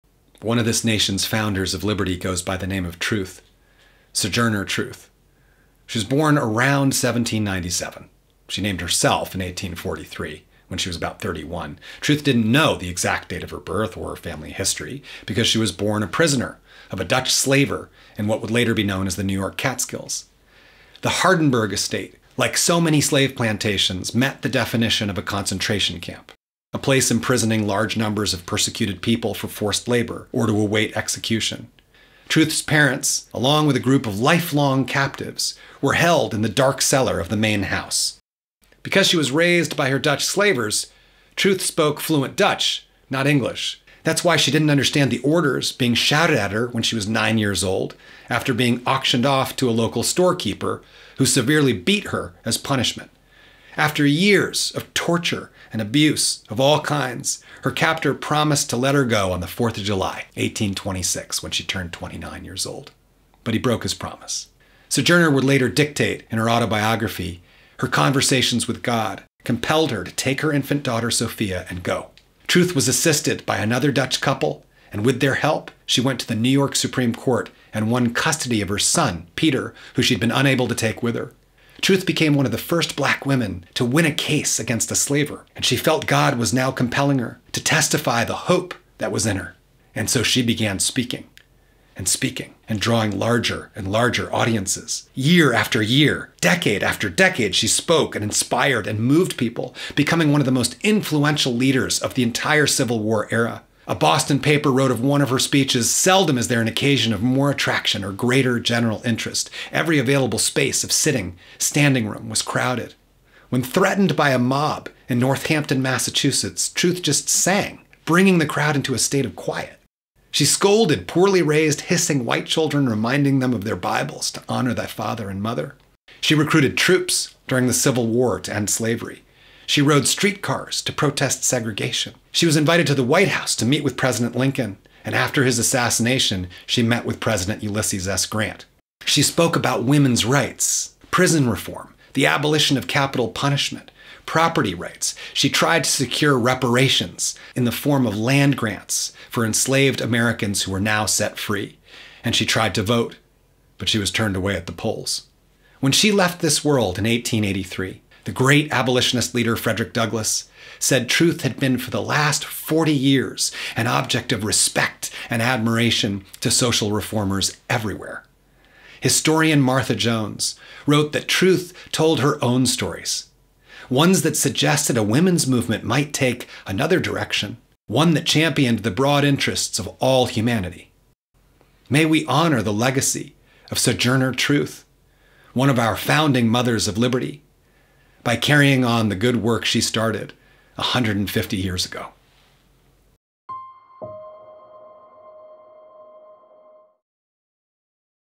Founding Mother of American Truth
She helped lead America to its 2nd Independence Day. She was an inspiration to social reformers everywhere. And she took the women's movement to a whole new level. Her name was Truth.